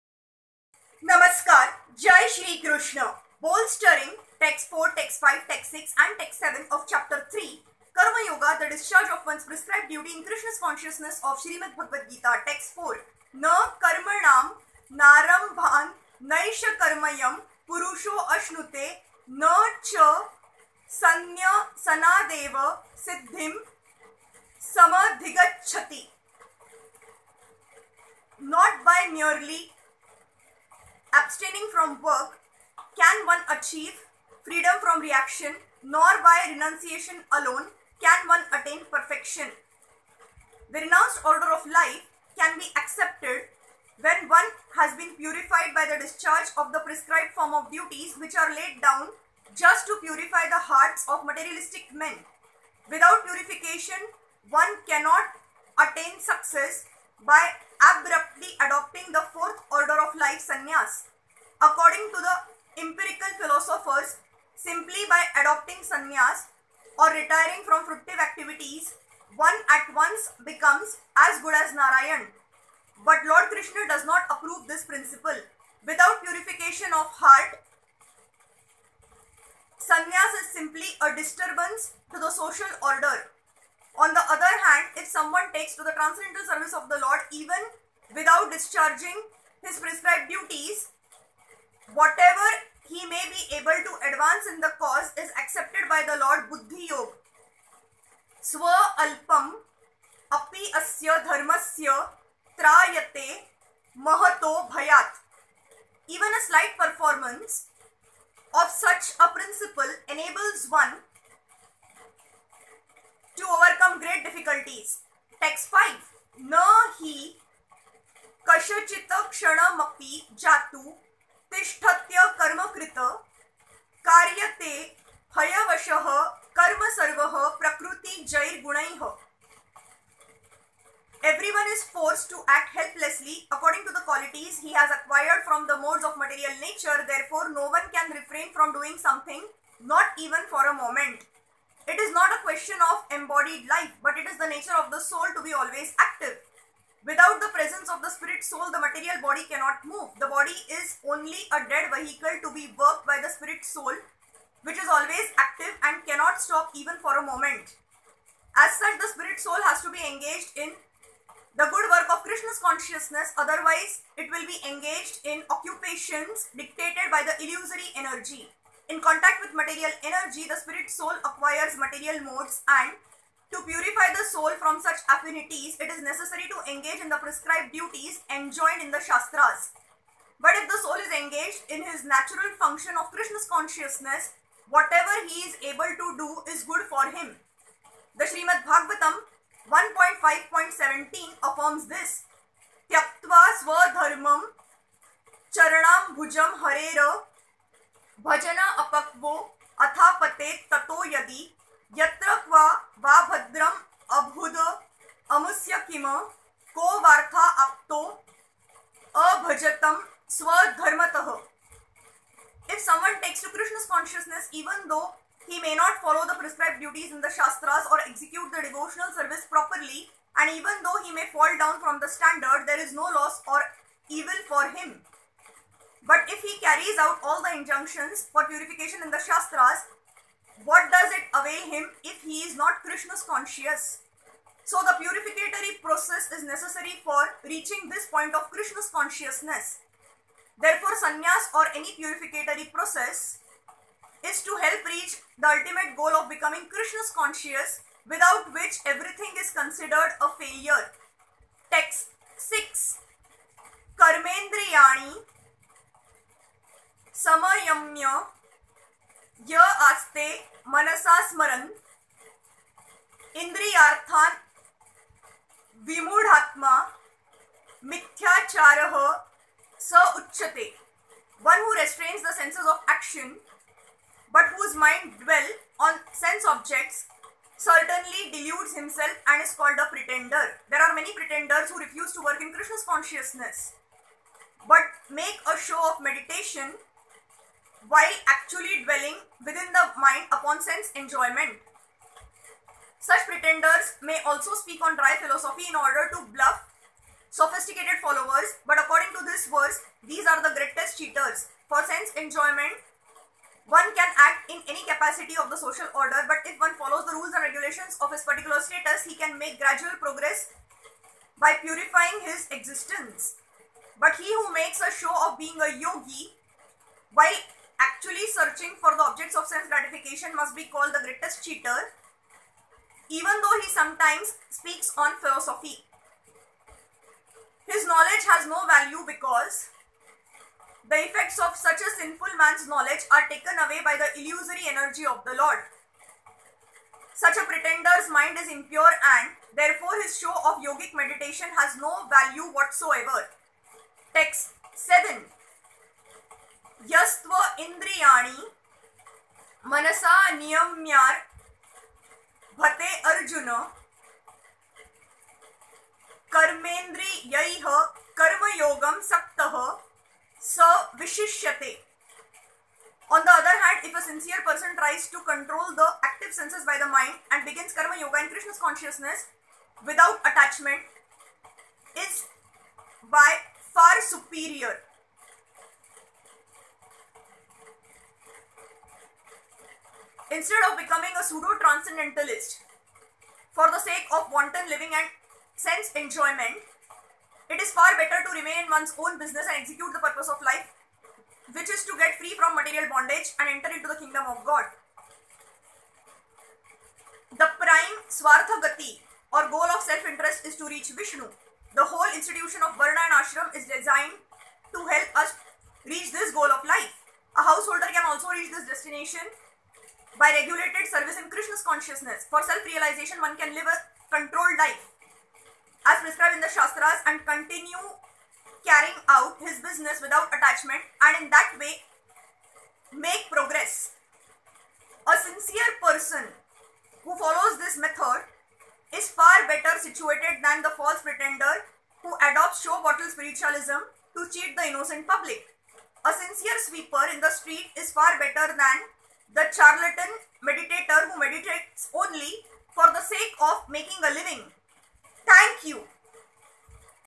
Texts 4,5,6 & 7 Chapter 3.Karma-Yoga:The Discharge of One's Prescribed Duty in Krsna Consciousness
Jai Shri Krishna Shrikrishna Vani Gita-gyan Krsna-Arjuna Samvad Chapter 3.Karma-Yoga:The Discharge of One's Prescribed Duty in Krsna Consciousness (Texts 4,5,6 & 7) of Srimad Bhagavad-gita "Hare Krsna,Hare Krsna,Krsna Krsna,Hare Hare/Hare Rama,Hare Rama,Rama Rama,Hare Hare." "Om Namo Bhagavate Vasudevaya Namah."